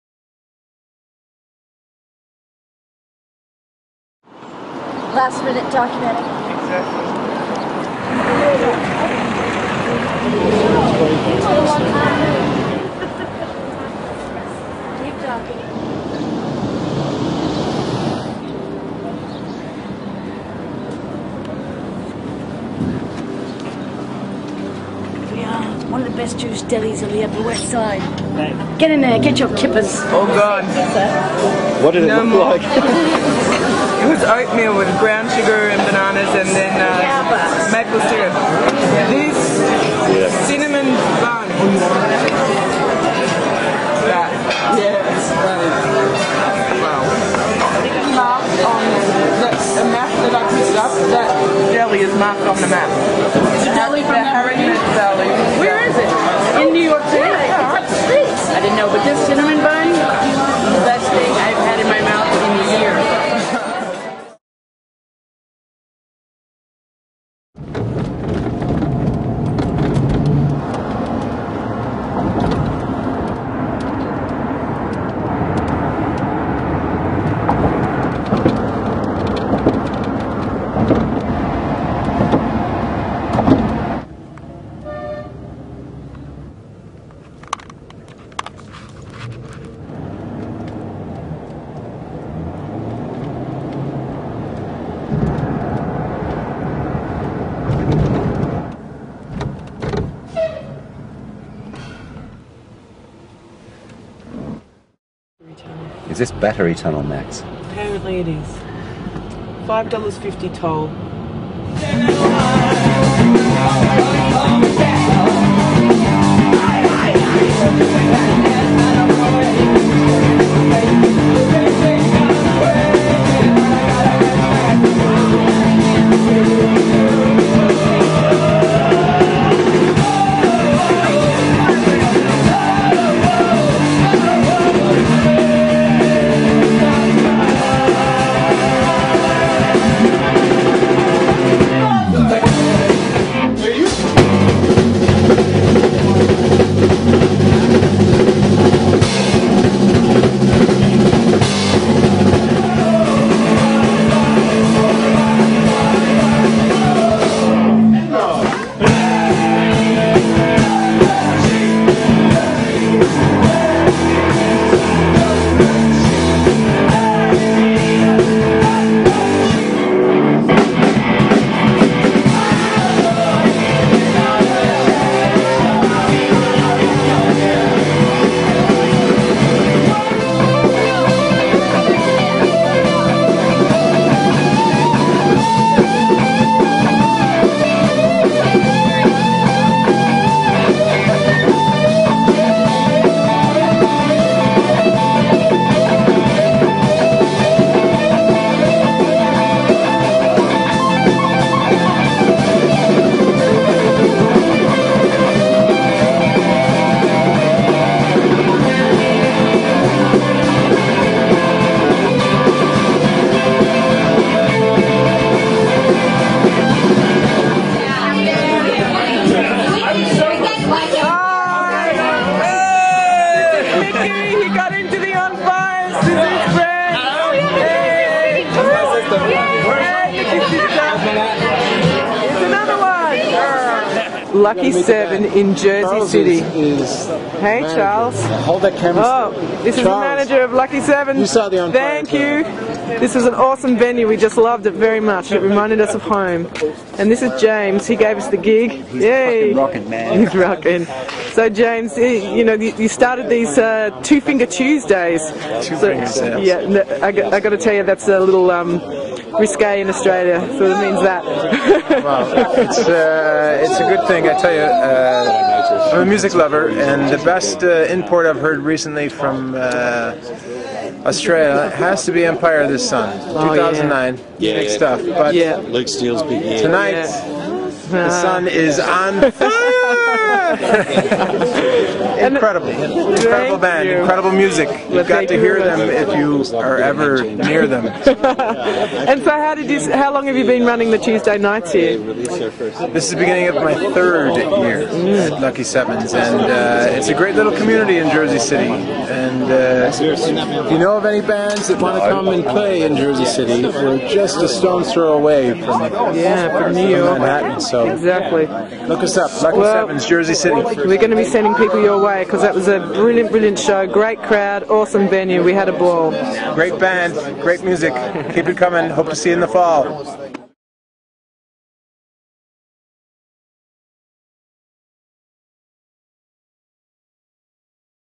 The On Fires USA tour vid part 5-08Mar10 LOW RES
The On Fires on tour in the US, Feb / Mar 2010. In this episode, we're in Manhattan, NYC 'doing' breakfast at Lansky's (famous deli) with Shaynee and Terese. We've been staying with them for a week by this stage. After that we head down to Brooklyn to stay, then onto a great show at Lucky 7 in Jersey City. That show was one of our tour favourites. A tiny packed bar, much like the Yak Bar in Melbourne (where we got our start). Face to face with the crowd, up close & personal - the way we like it.

Episode 6 coming soon: WARY FM, New York and travelling upstate to Ithaca.